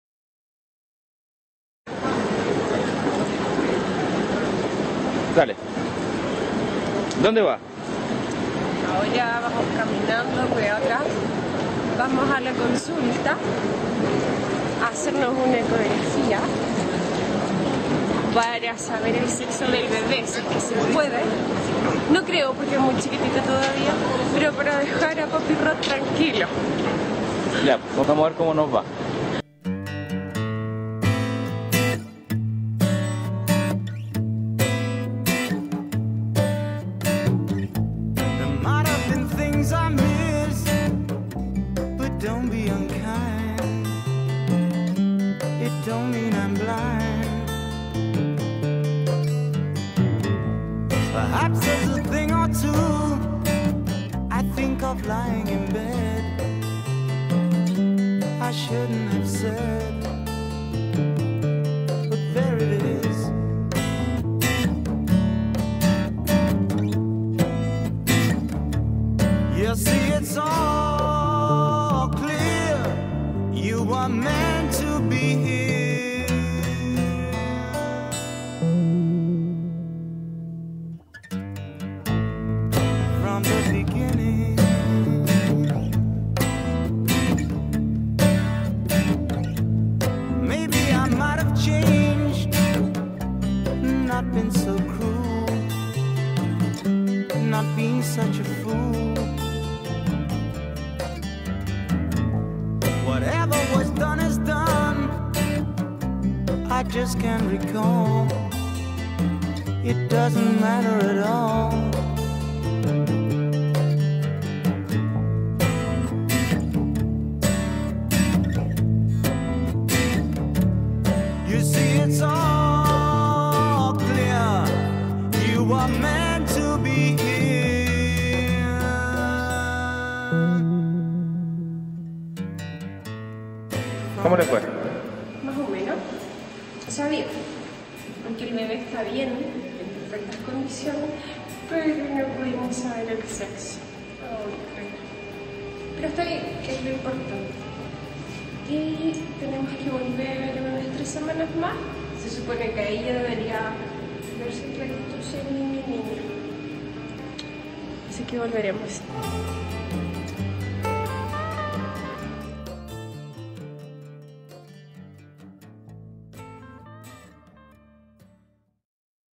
16 semanas
Nuestro bebé a las 16 semanas. Nino o niña?? aun no se sabe...